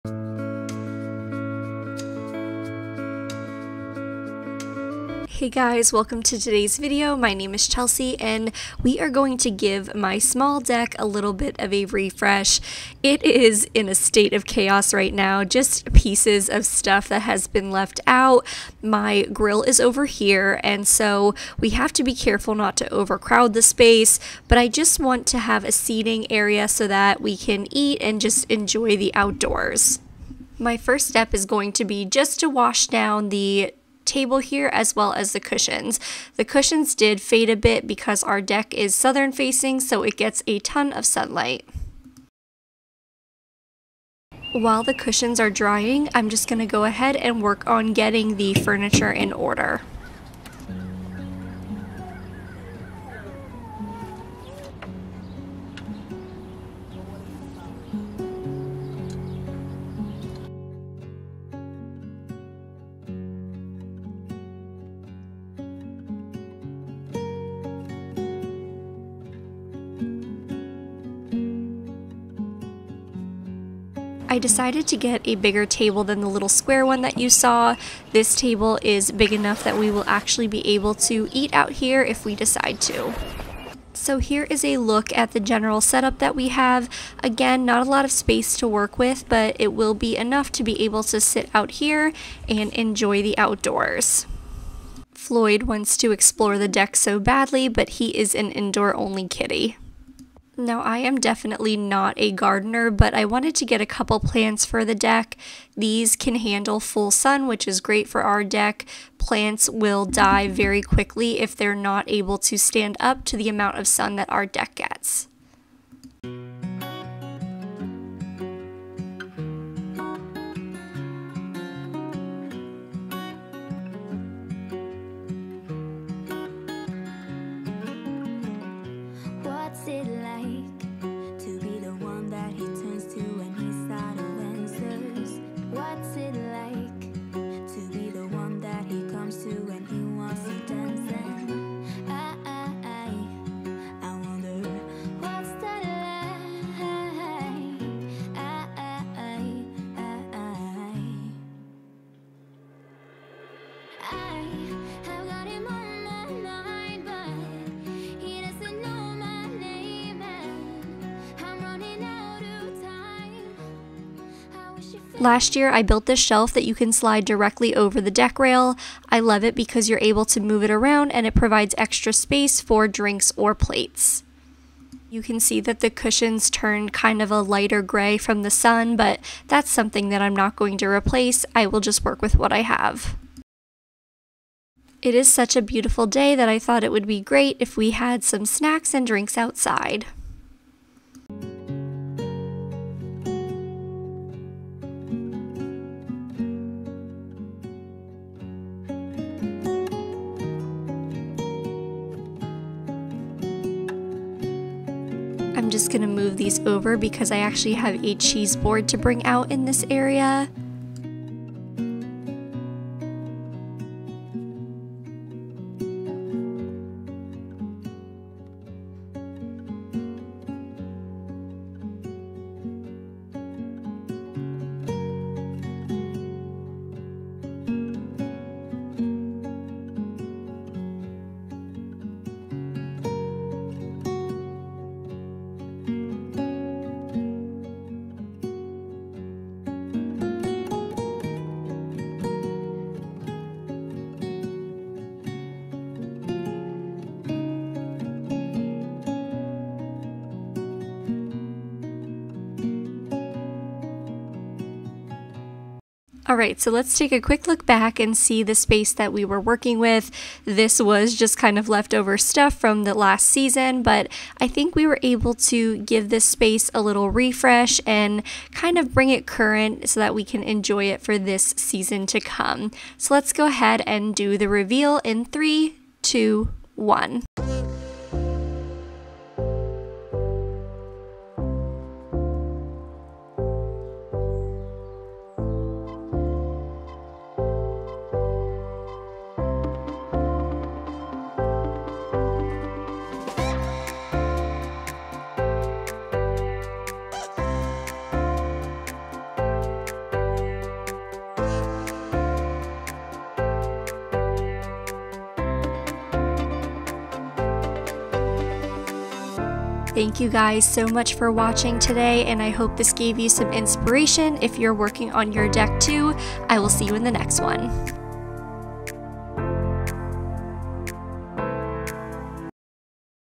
Small Deck Refresh | Small Deck Decorating Ideas
Hello everyone! In today’s video I am going to be giving my small deck a refresh. I hope you are able to get some ideas for your home too! Working with a small amount of space can be challenging, but doesn’t mean you still can’t have a nice place to enjoy the outdoors.

LINKS
Chair set